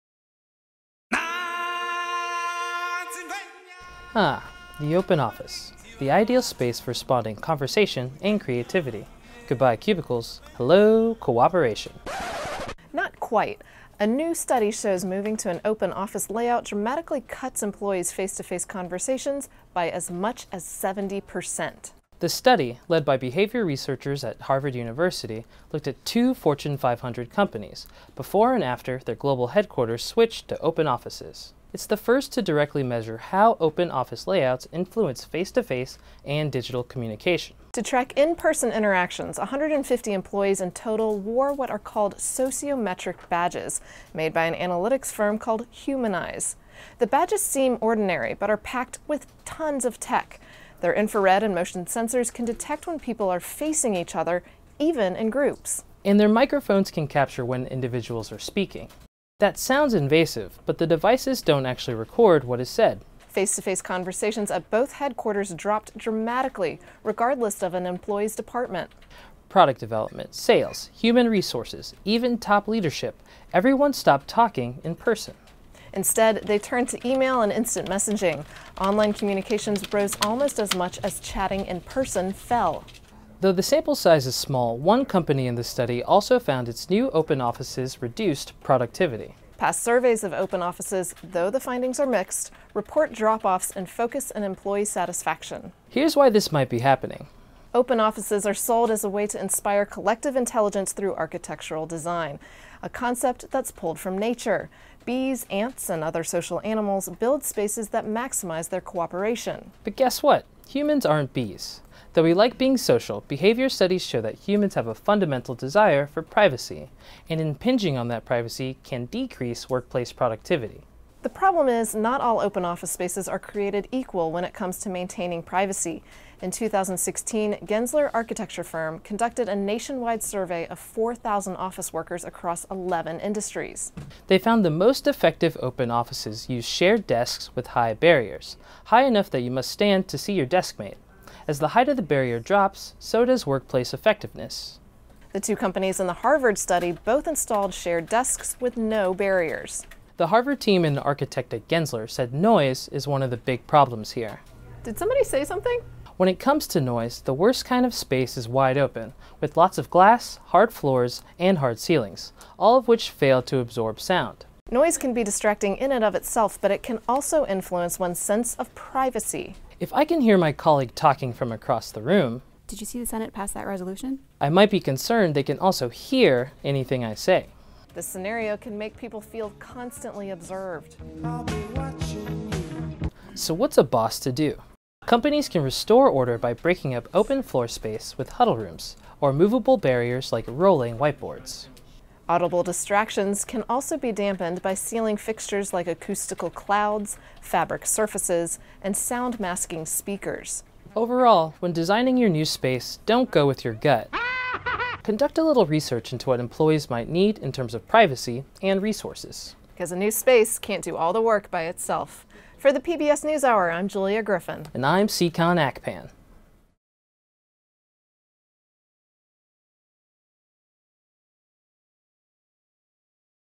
Here’s proof that open office layouts don’t work, and how to fix them
The basic logic behind the open offices is that tearing down physical barriers inspires communication and collective creativity.

But there is little evidence to support these widespread claims — and some surveys show the opposite: declines in employee satisfaction and productivity.

Now, two behavior researchers from Harvard University have tackled this problem head on, by directly measuring more than 100,000 face-to-face conversations at two Fortune 500 companies — before and after their global headquarters switched to open office layouts.

The team found open office layouts dramatically cut employees’ face-to-face conversations at these companies — by as much as 70 percent.